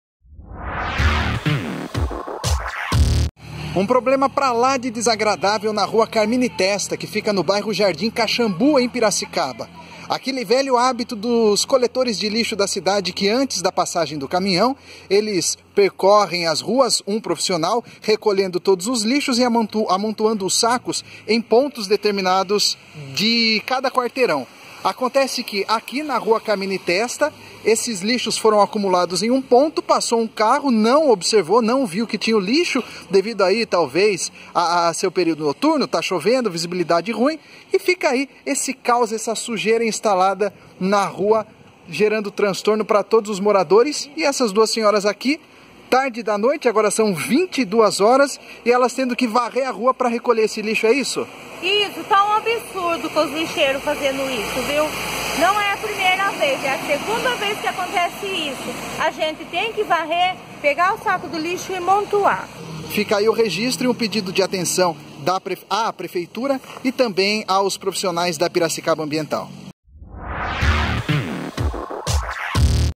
Durante a chuva motorista bate em lixo e espalha sujeira no Caxambu
Durante a chuva de ontem (terça-feira), um carro bateu em uma 'montanha' de lixo no Jardim Caxambu.

O motorista não parou, mas deixou um rastro de sujeira dando muito trabalho aos moradores.